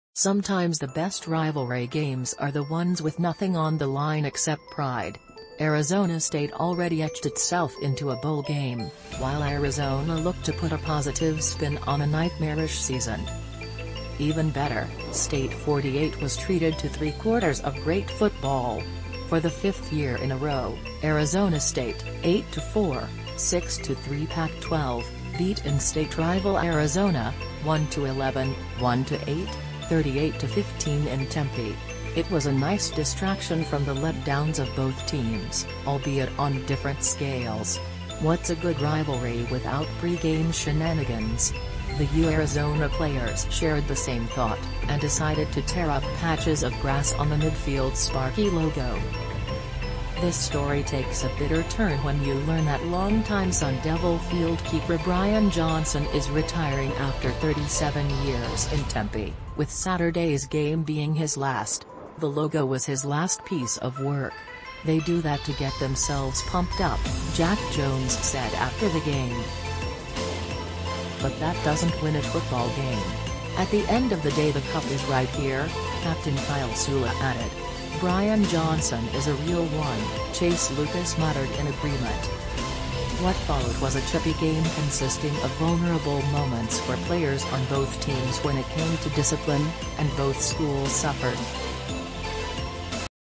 ASU Football Sun Devils win Territorial Cup for fifth year in a row
Sometimes the best rivalry games are the ones with nothing on the line except pride. Arizona State already etched itself into a bowl game, while Arizona looked to put a positive spin on a nightmarish season. This year’s Territorial Cup had no conference or postseason implications, allowing emotions to pile up and flow freely in a way that makes football fans and players go primal. Even better, State-48 was treated to three quarters of great football. For the fifth year in a row, Arizona State (8-4, 6-3 Pac-12) beat in-state rival Arizona (1-11, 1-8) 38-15 in Tempe. The unsportsmanlike penalties flew on nearly each drive. There was an ejection, as well as a ceremonial stomping-of-the-logo. The rivalry-buzz lingered throughout the stadium. It was a nice distraction from the let downs of both teams, albeit on different scales. What’s a good rivalry without pregame shenanigans? The UArizona players shared the same thought, and decided to tear up patches of grass on the midfield Sparky logo. This story takes a bitter turn when you learn that longtime Sun Devil field-keeper Brian Johnson is retiring after 37 years in Tempe, with Saturday’s game being his last. The logo was his last piece of work.“They do that to get themselves pumped up,” Jack Jones said after the game. “But that doesn’t win a football game.”“At the end of the day the cup is right here,” captain Kyle Soelle added.“Brian Johnson is a real one,” Chase Lucas muttered in agreement. What followed was a chippy game consisting of vulnerable moments for players on both teams when it came to discipline, and both schools suffered. Five unsportsmanlike conducts were accepted (more were called), a Wildcat player was ejected, and there were skirmishes between plays frequently.“Emotional fouls, going into this game we only had seven,” Herm Edwards said. “And we got a couple tonight, but in this game, if you’re a head coach, you chalk off three before the game starts. Cause it’s gonna happen. You tell them all week ‘don’t do it,’ at halftime ‘don’t do it,’ and then Jack (Jones) doesn’t only do it, I mean he really does it.”More on that in a bit, back to football. From the kickoff, it was apparent that this was not going to be a 70-7 slaughter. College football rarely is. Arizona pulled out all the tricks to take its first two drives for 77 and 67 yards, respectively, but only netted two field goals. Down three starters and a backup (Fields/Robertson/Butler/Markham), the ASU defense bent, but did not break. Meanwhile, the Sun Devils responded with a 75 yard-drive of its own, concluding with a 14-yard touchdown connection from Jayden Daniels (10-14 / 86 yards / two TDs) to Ricky Pearsall (five catches / 52 yards / two TD). Coming into this game, Daniels had one touchdown pass in November 2021. He doubled that total after finding Pearsall again later for their second combined touchdown of the afternoon. Up 7-6, ASU punted after a quick three-and-out.